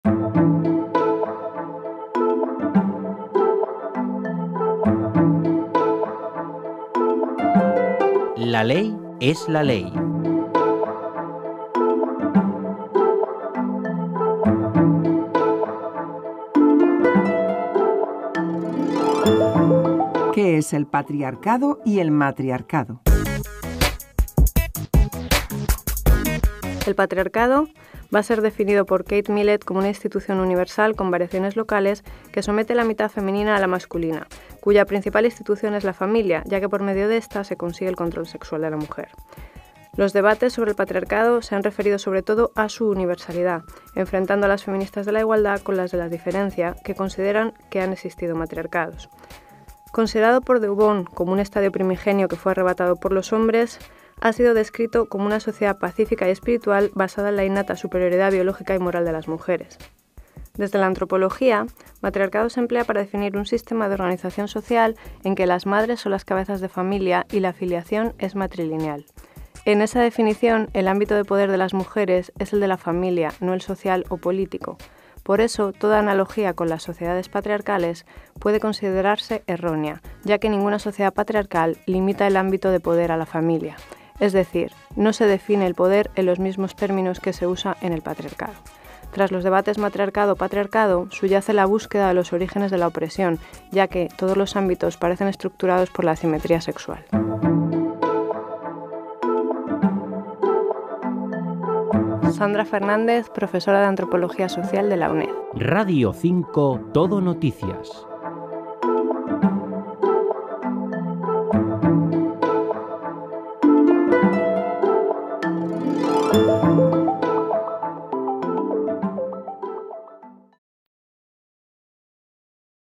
¿Qué es el patriarcado? ¿y el matriarcado?
Serie: La Ley es la ley

Las definiciones de patriarcado y matriarcado son variables histórica e intelectualmente. Para dar una definición amplia podríamos decir que es una institución política en la que un género ejerce fuerza, dominio o control sobre el otro. Los antropólogos evolucionistas emplearon los términos en referencia al tipo de derecho (paterno o materno) en una forma de reconstrucción histórica ficcional. Más tarde Malinowski lo empleará para aludir a la distribución de poder. Tras los debates matriarcado-patriarcado subyace la búsqueda de los orígenes de la opresión, ya que todos los ámbitos parecen estructurados por la asimetría sexual.

Participan:
 Sandra Fernández García profesora de Antropología Social, UNED